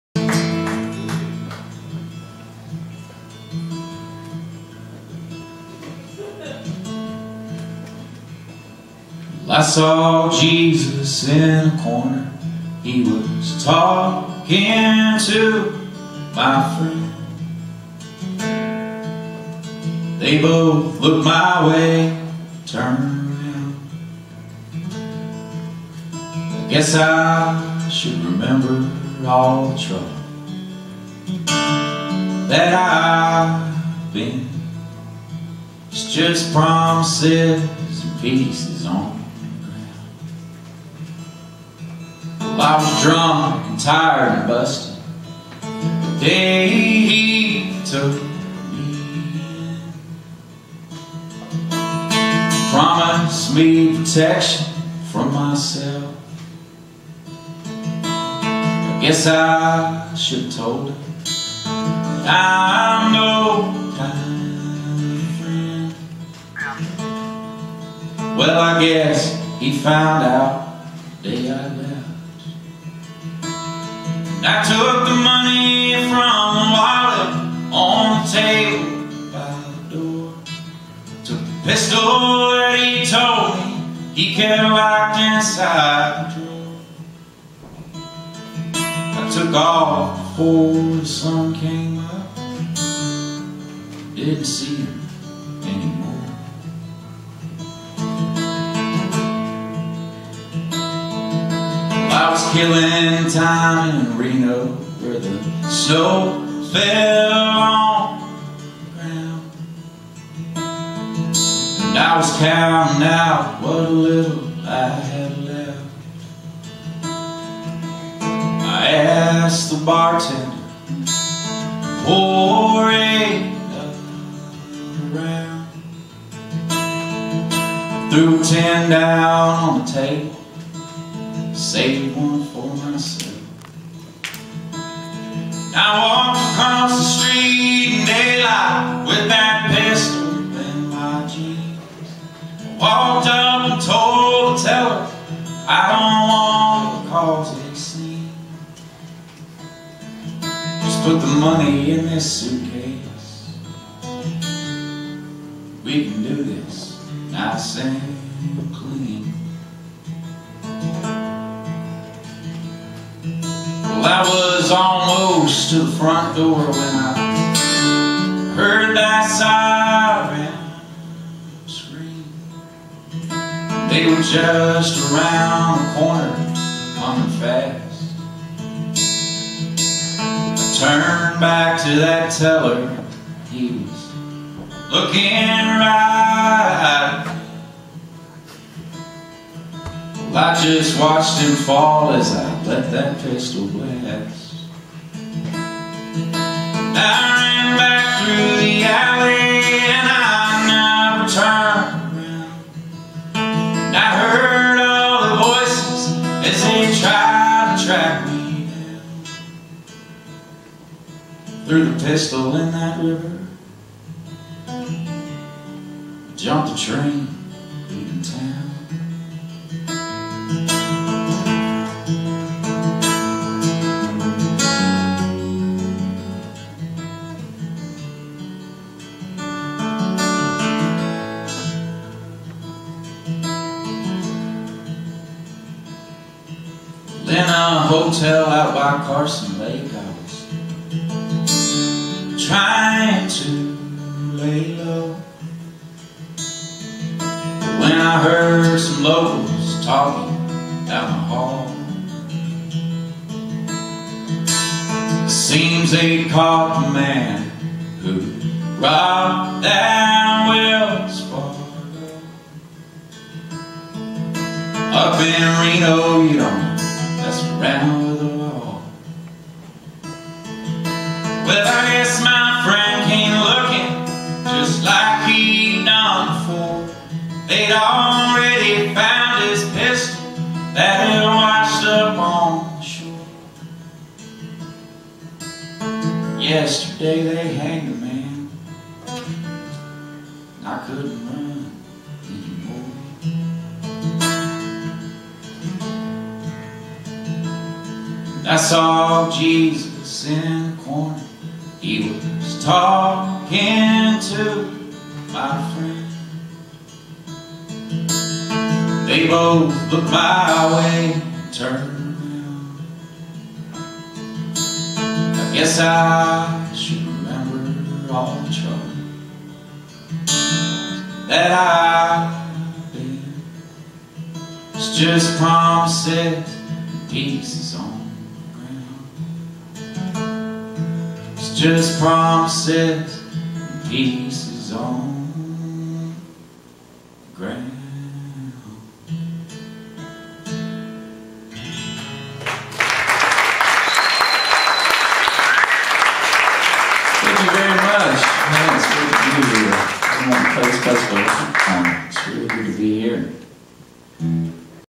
Jason Eady at The Kessler Theater in North Oak Cliff
Recorded live on November 1, 2013 at the historic Kessler Theater in X+/North Oak Cliff.